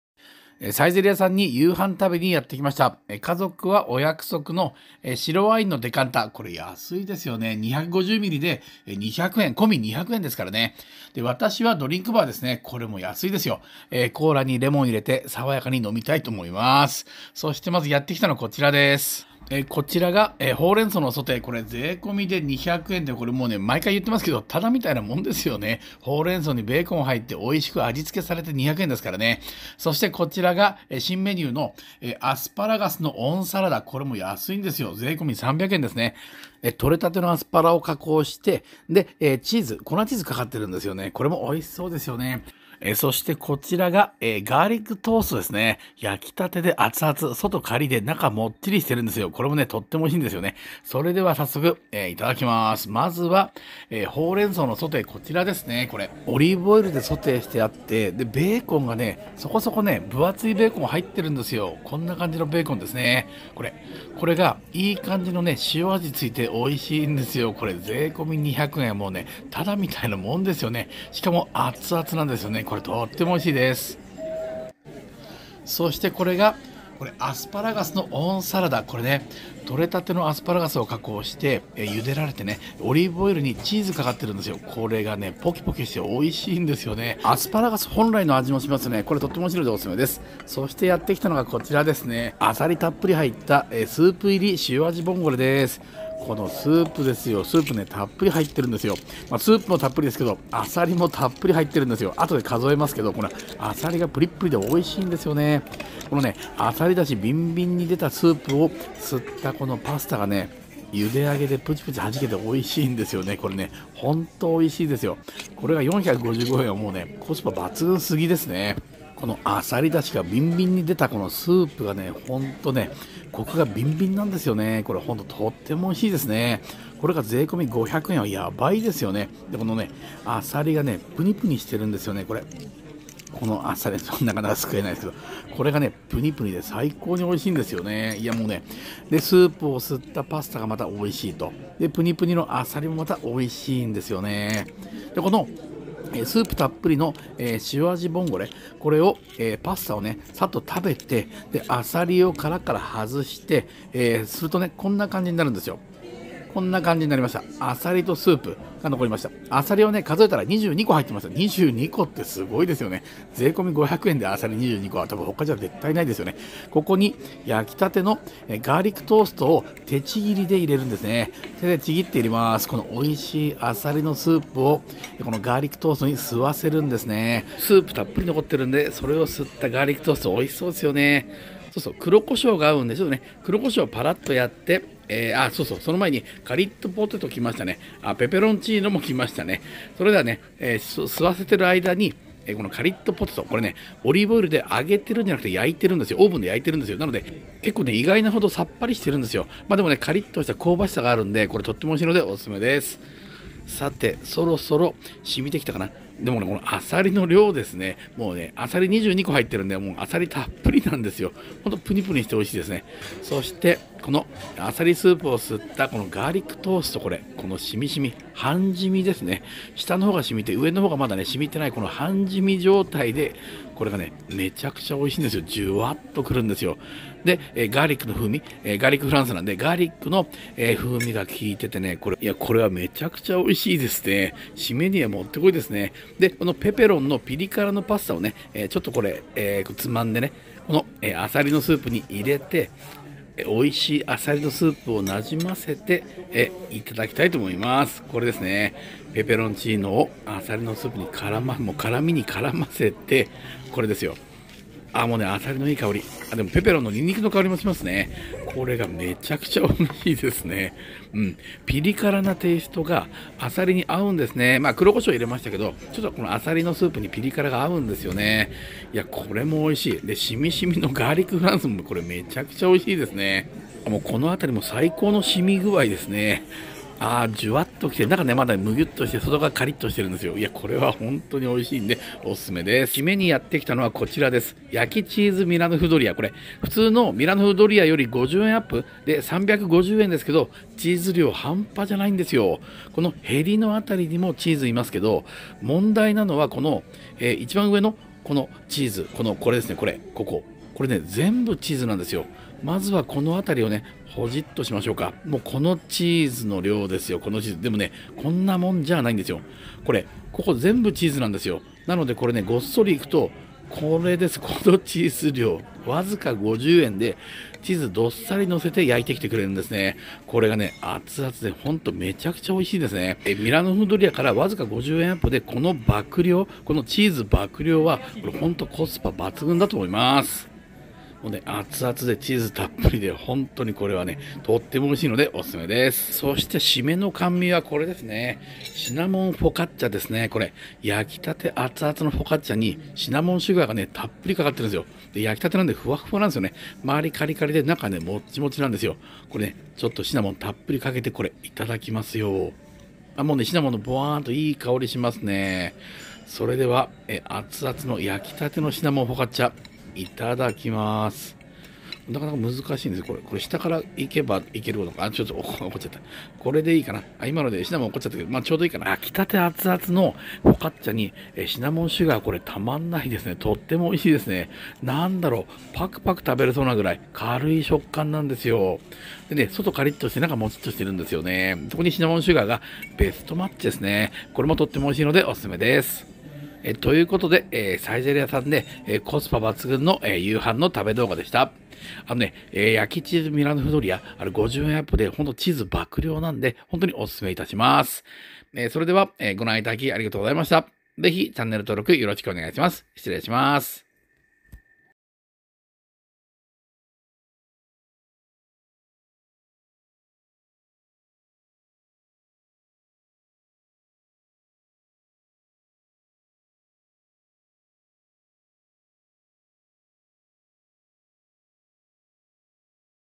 サイゼリヤさんでコスパ抜群晩ごはん
ご覧頂きありがとうございますm(__)m

サイゼリヤさんで晩ごはんを食べてきました。

相変わらずコスパ抜群のサイゼリヤさん。
あさり22個入ったスープ入り塩味ボンゴレはあさり出汁ビンビンのスープが絶品の逸品にして込500円というコスパ最強ぶり。
そのあさりスープに焼き立て熱々のガーリックフランスをちぎり入れて染み染みにするとあさりの旨味がジュワァと染み出てガーリックの香ばしさと相まってとても美味しいアレンジでした。